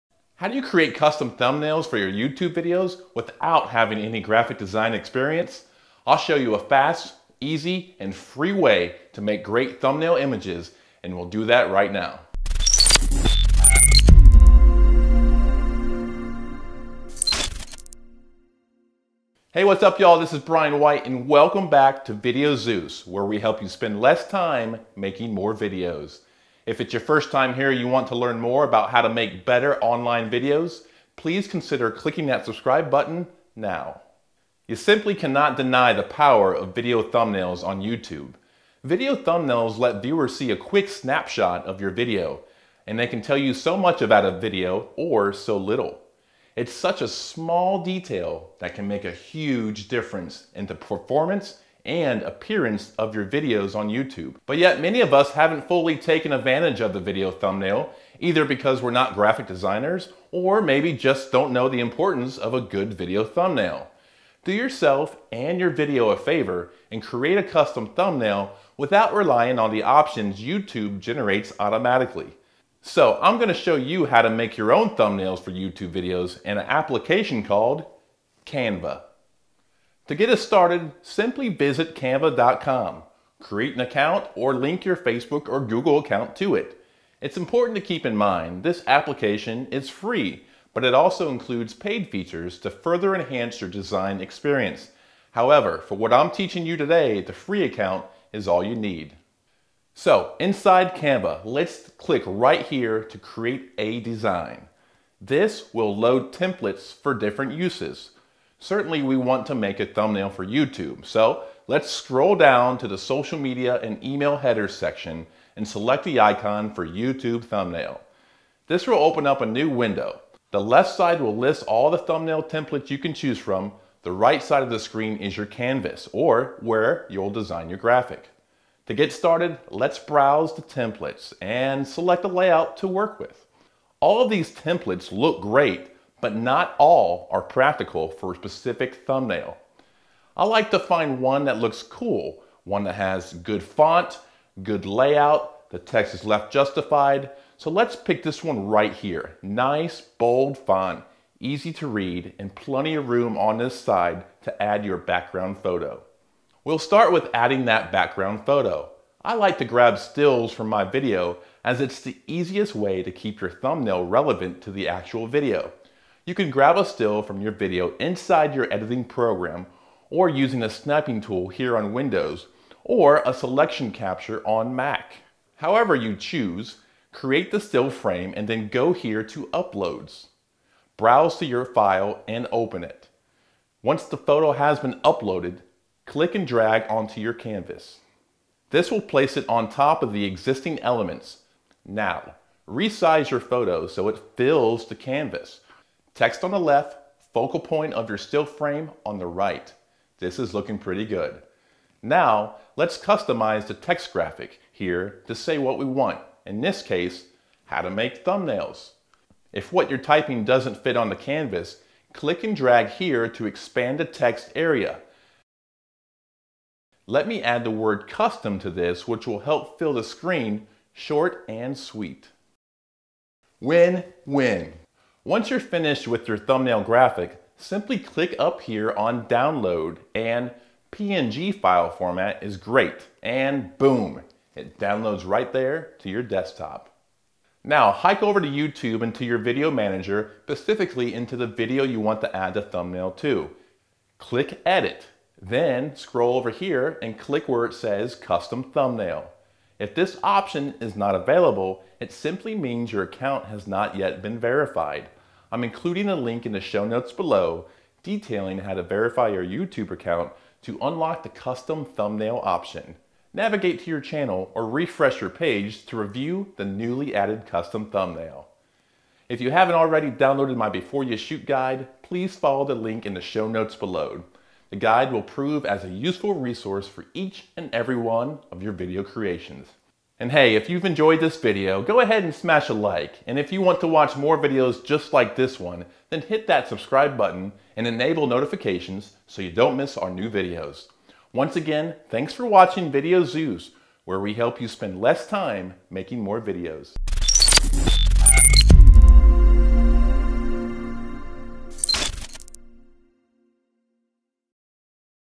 YouTube Custom Thumbnails
How do you create YouTube custom thumbnails for your videos without having any graphic design experience? I'll show you a fast, easy, and free way to make great custom thumbnail images on Canva.

Custom YouTube thumbnails can make or break your video in search listings. It's an important item that helps viewers decide whether to watch you video or not. So grab your note pad as we dive into how to make custom thumbnails on YouTube - fast, easy and free!

The proper size for YouTube thumbnails is 1280x720 pixels in 72 dpi.

Looking to verify your YouTube account? Follow the link below to verify your account to enable the custom thumbnails option.

Looking for a better way to make videos with your phone? Download the Moment Pro Camera app then watch our video tutorial.